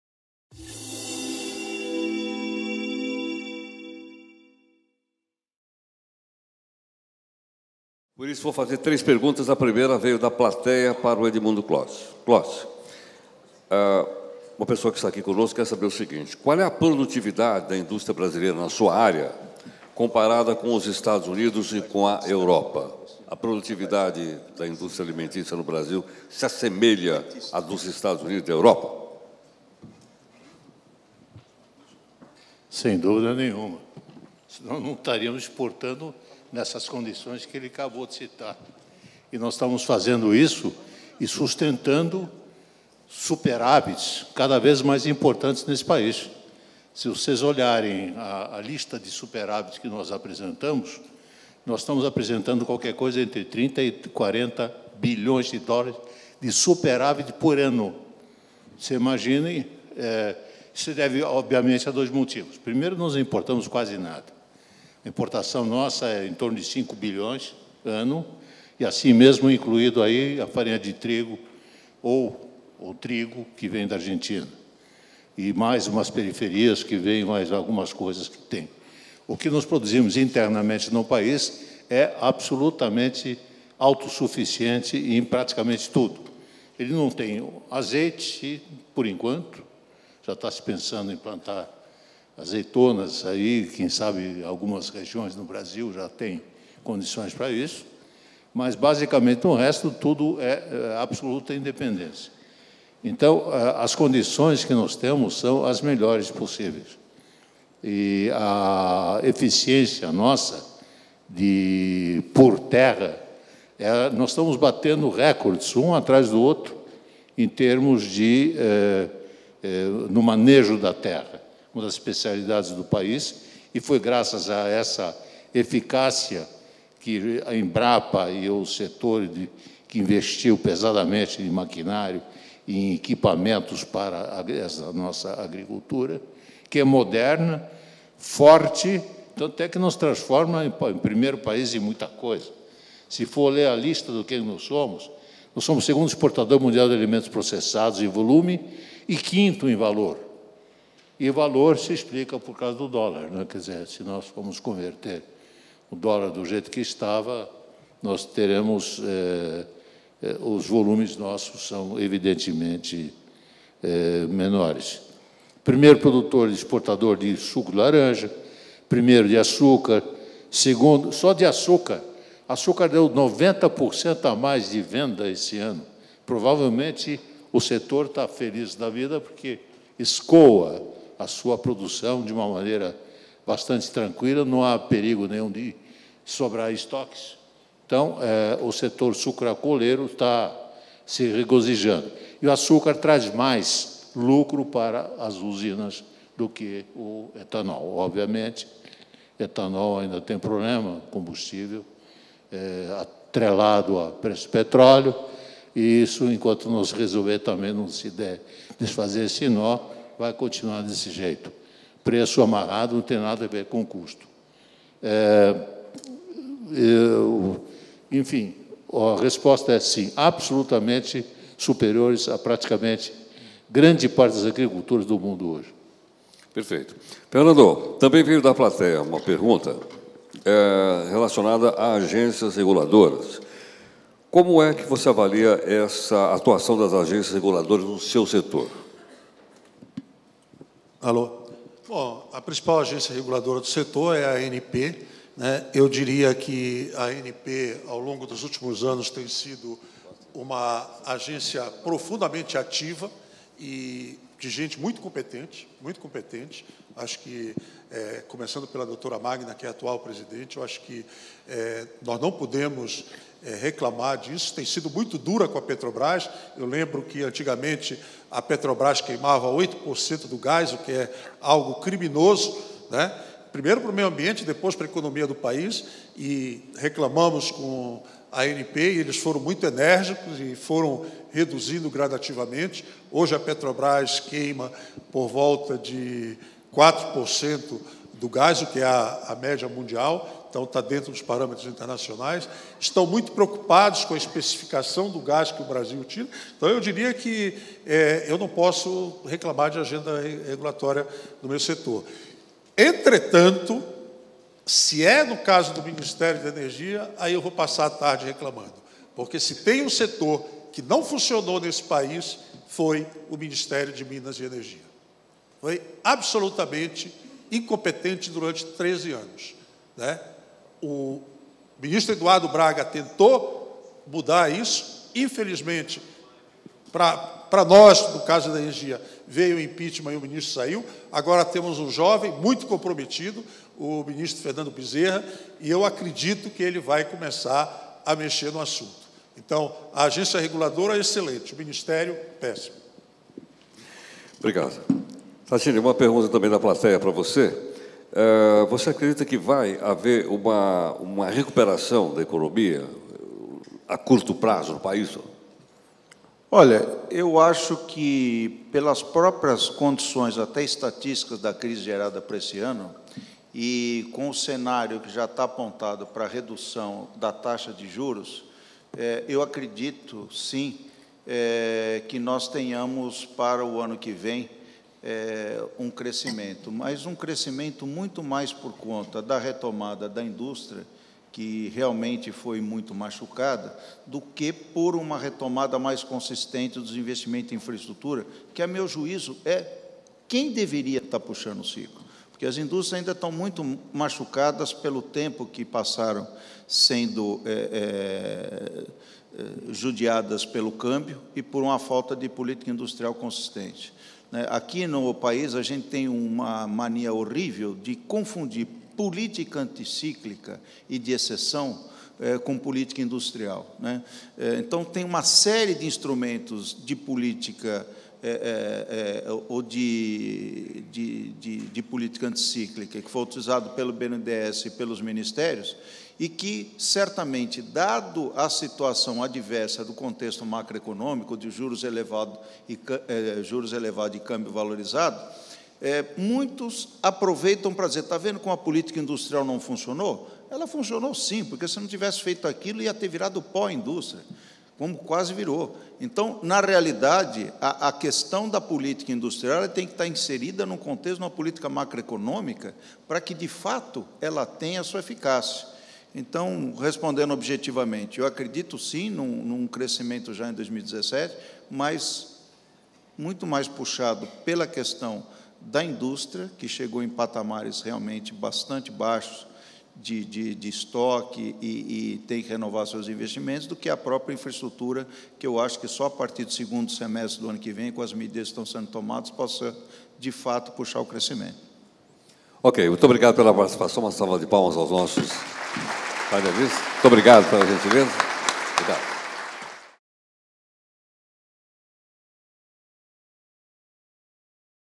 Painel: Perspectivas para grandes setores da economia nacional
O Seminário Perspectivas Brasil 2017, realizado na sede da Amcham Brasil, discutiu o que esperar para os setores químico, alimentício e de infraestrutura.

Participaram do Painel:
- Edmundo Klotz, presidente da Associação Brasileira das Indústrias da Alimentação
- Fernando Figueiredo, presidente da Associação Brasileira da Indústria Química
- Venilton Tadini, presidente da Associação Brasileira de Infraestrutura e Indústrias de Base